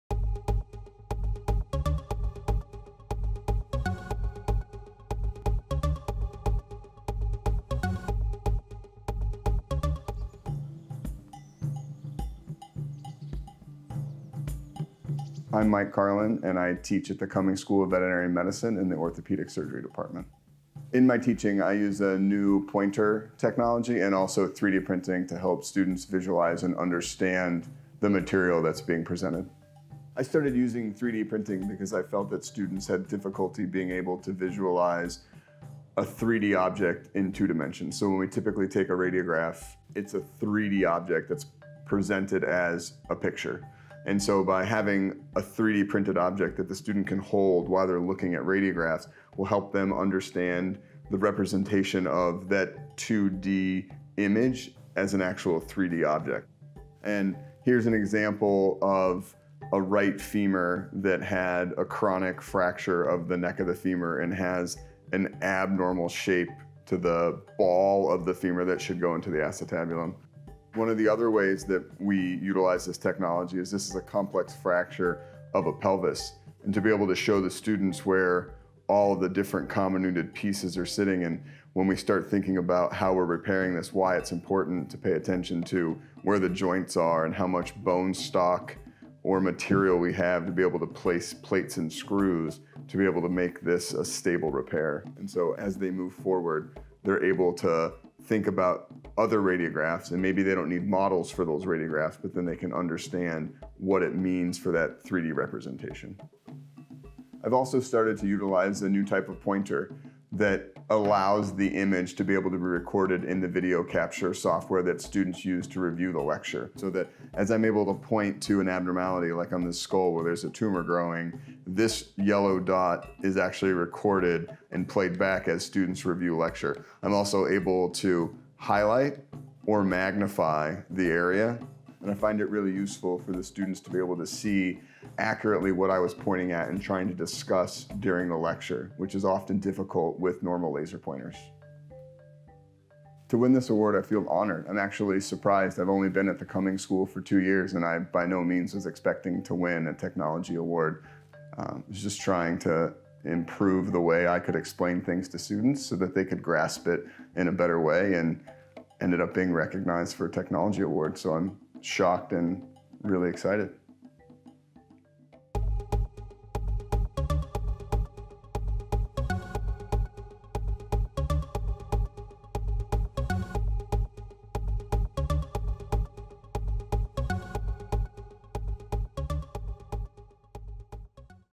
Mike Karlin - 3D printing to help with visualization
Using new pointer and 3D printing to help students learn through visualization